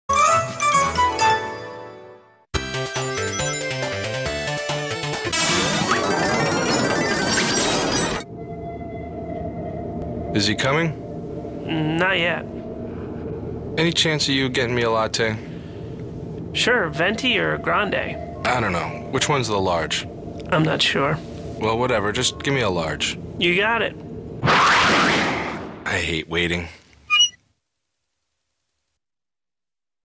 A Day in the Extra Life: Sonic - Latte
It isn't all fun and games for Dr. Robotnik. There's a lot of waiting involved.